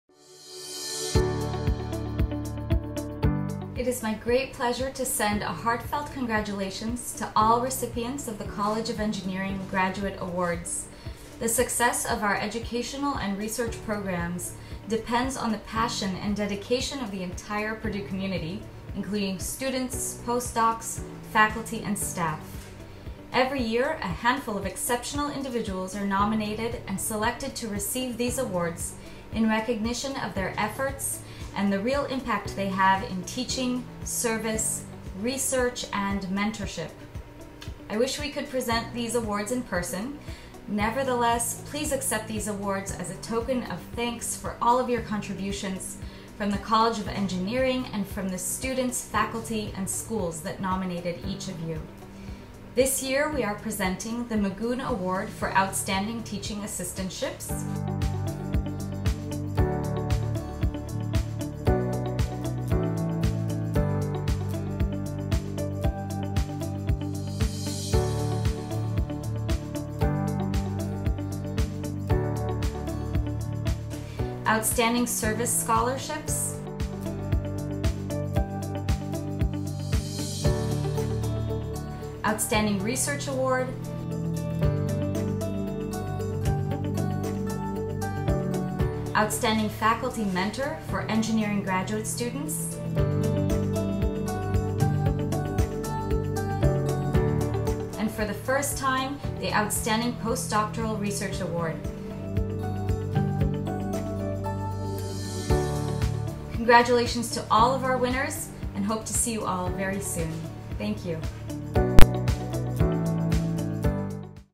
2019-2020 Engineering Graduate Program Awards
Every April, the Engineering Graduate Program recognizes the graduate students who excel in teaching, service, and research. Even though we can be together for this year's awards, we hope this video honors the graduate program awardees, faculty mentors, and a new postdoctoral researcher awardee, along with a special message from Associate Dean of Graduate Education, Dana Weinstein.